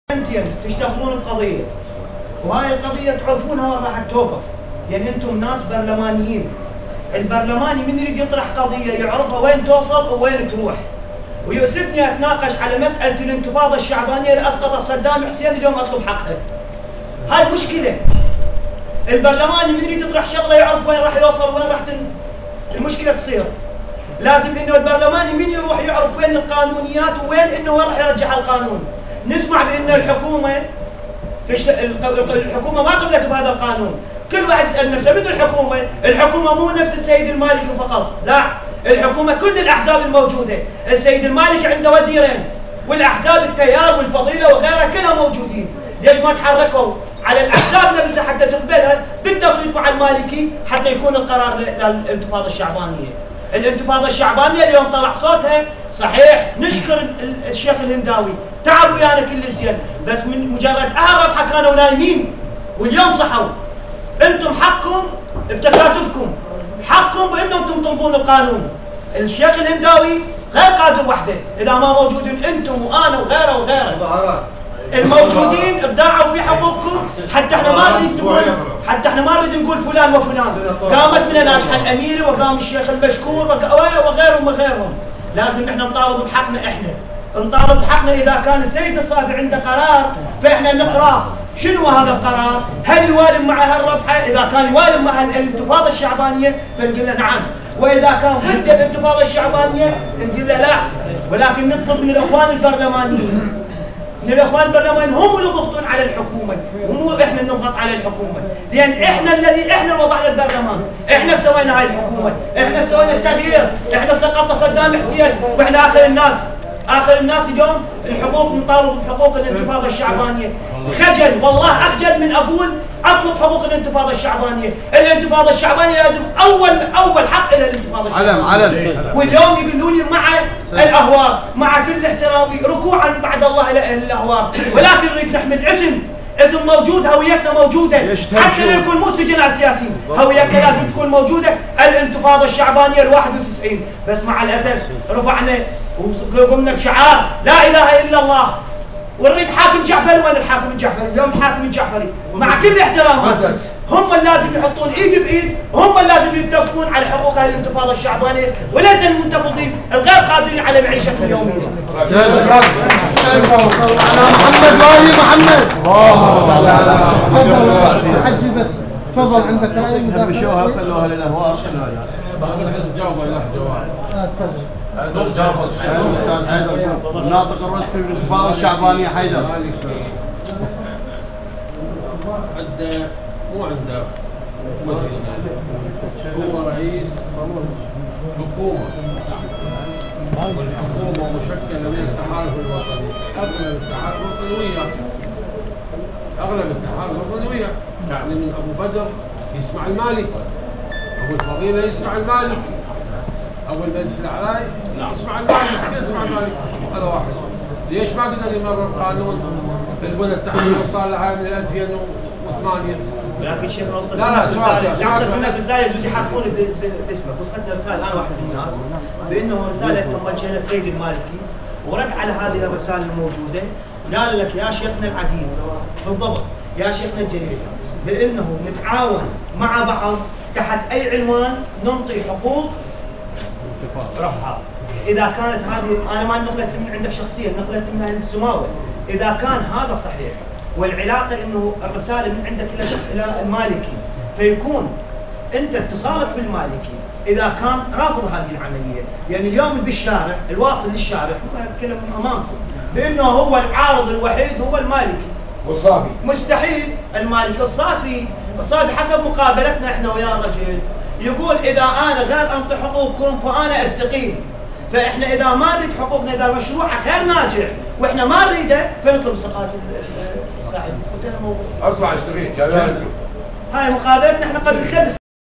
الانتفاضة الشعبانية
الاخ حيدر جبار احد ابناء رفحاء يطالب الشيخ محمد الهنداوي بحقوق ابناء الانتفاضة الشعبانية المباركة